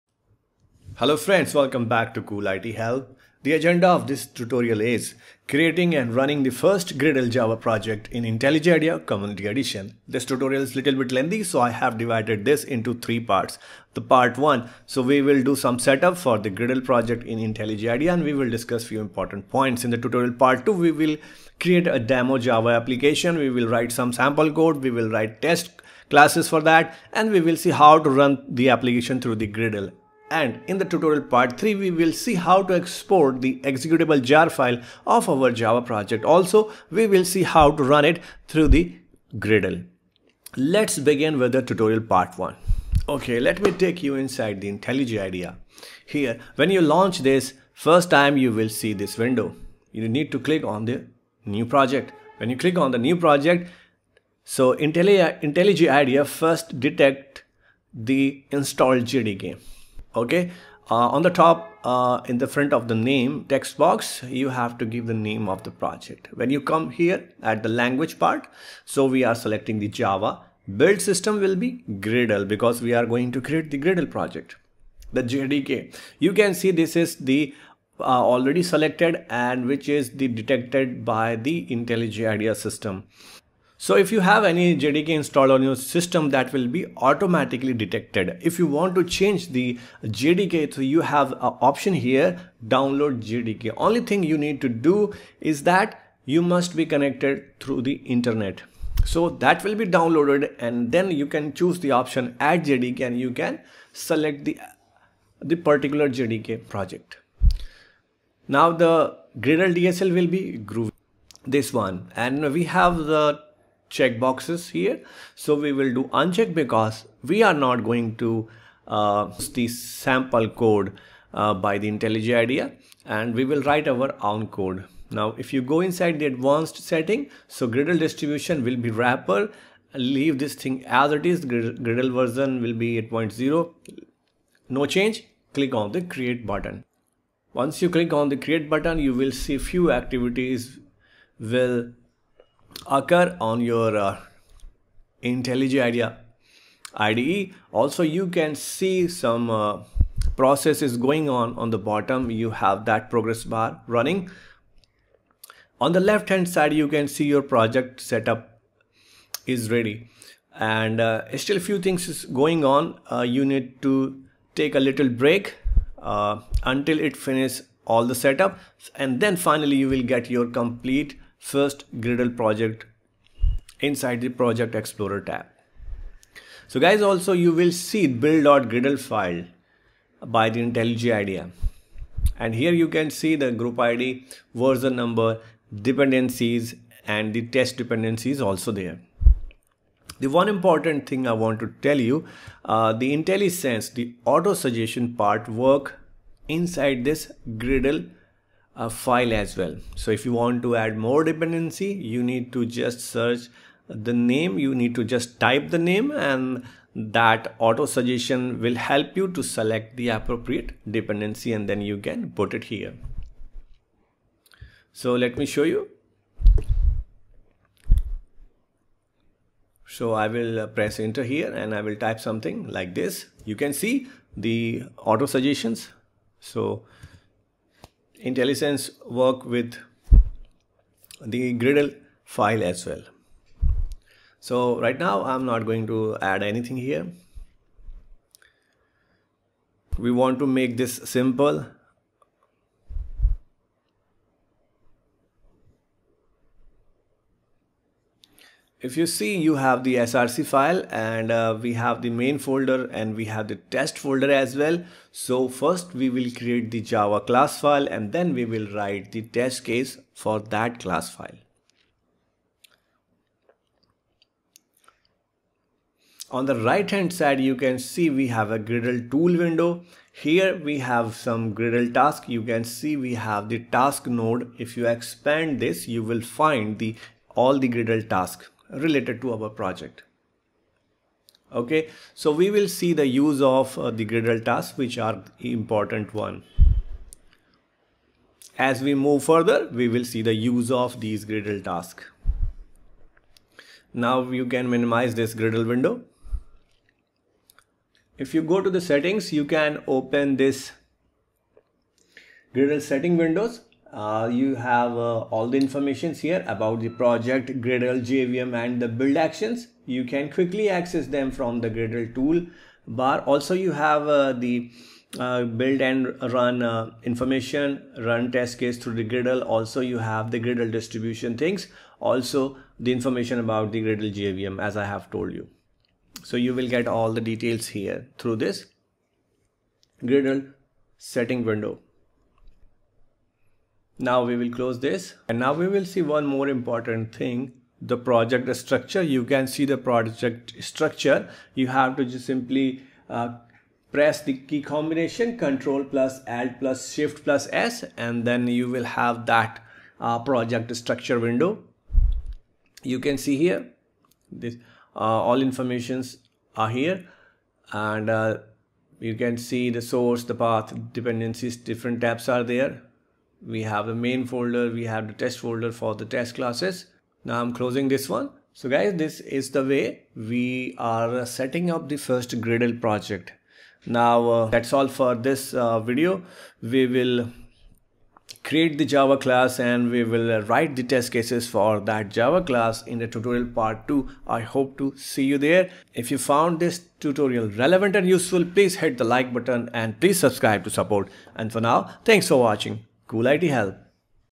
Create and run the first Gradle java project in IntelliJ Idea Community Edition | Tutorial Part-1
This is tutorial part-1. In this tutorial, we will see how to create and run gradle java project in the IntelliJ IDEA community edition. I tried to explain everything in detail for better understanding for beginners.
* Create/set up a Gradle project with Java and discuss on few additional important/useful points.